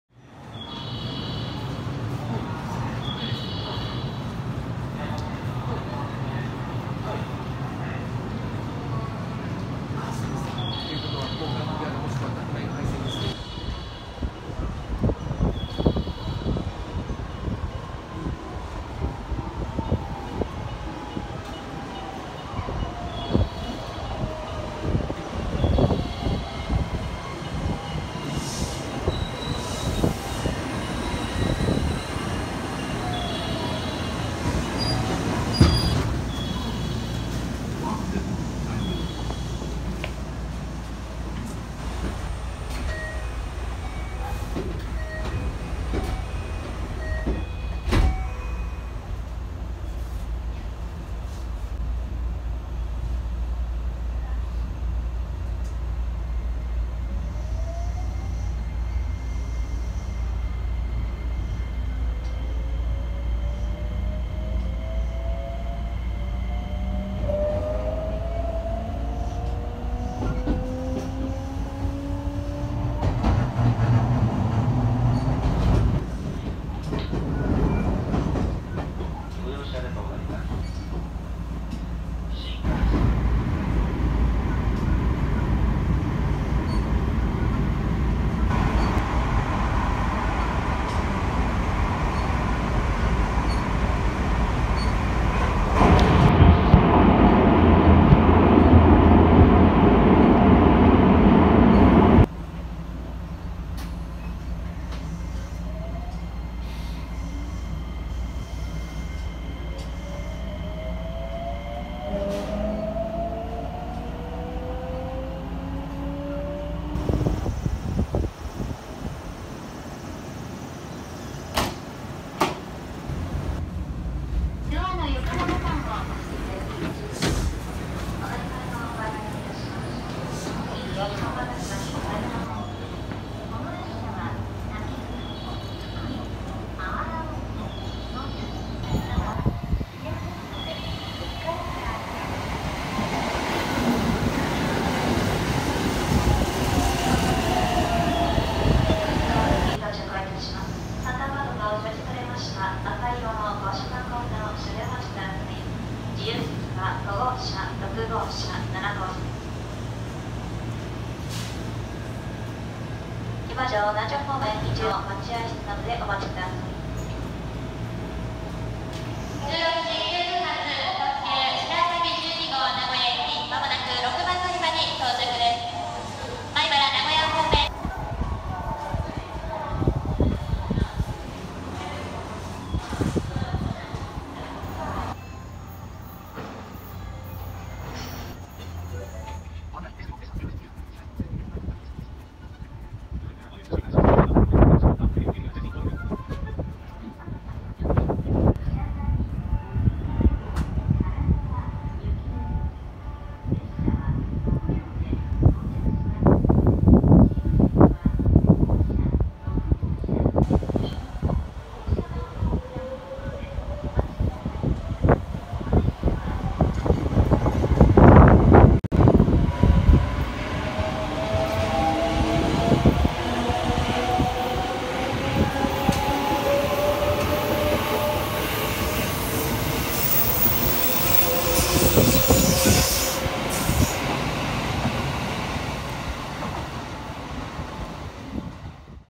新快速の行き先にある「敦賀駅」はこんな駅です【駅探訪 01駅目】
最近は新快速ネタが多めです

2006年より新快速が乗り入れを開始した敦賀駅。これにより福井県内→姫路まで乗り換え無しで行く事ができるようになりました。(因みに福井県民の間での「新快速」の知名度は割と低いです)